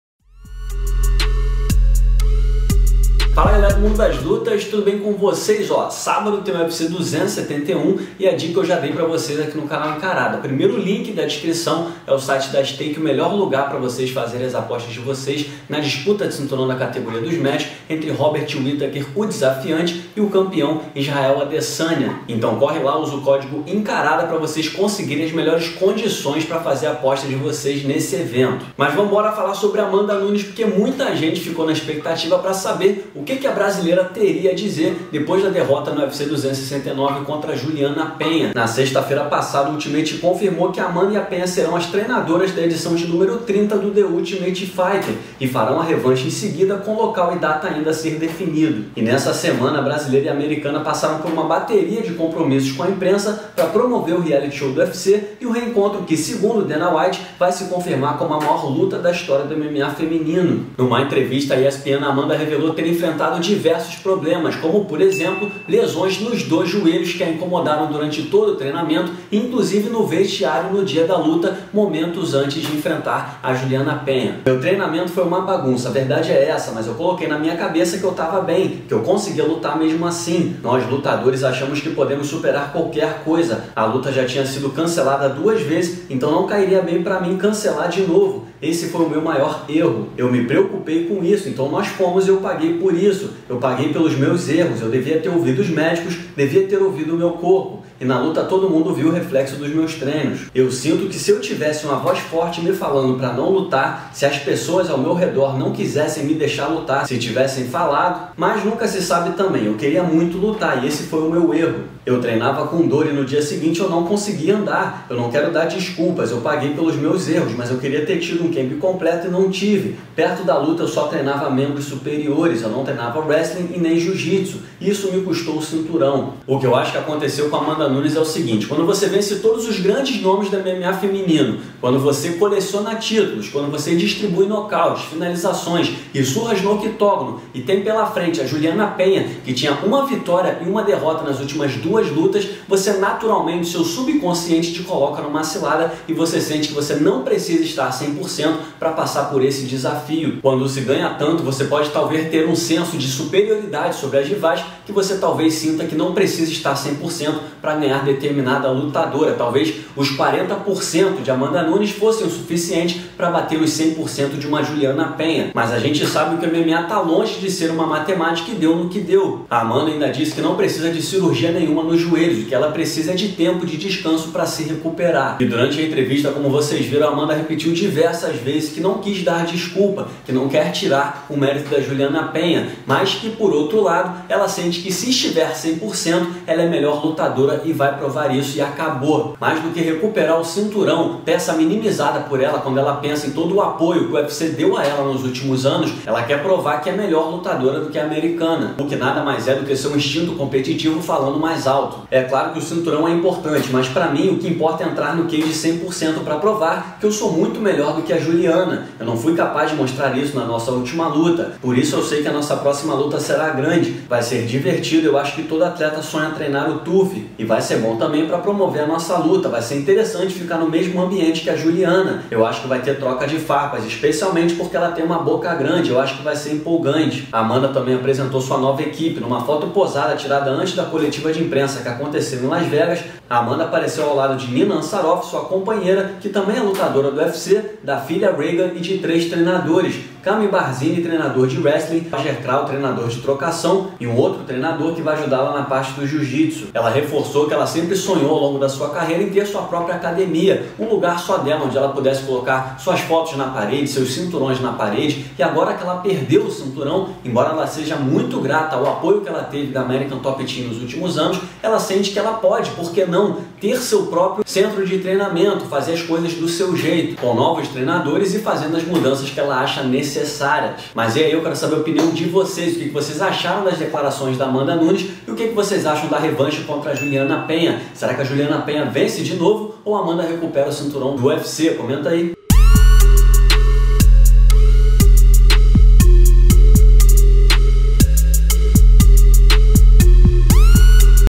AMANDA NUNES REVELA QUE SOFREU COM PROBLEMA ANTES DE DERROTA NO UFC
COD: ENCARADA

Fala, galera do mundo das lutas! O que vocês acharam das declarações de Amanda Nunes? Quem vence a revanche? A brasileira ou Julianna Peña? Comenta aí!

Sorriu? LARGA O LIKE!
Perguntas? COMENTE AÍ!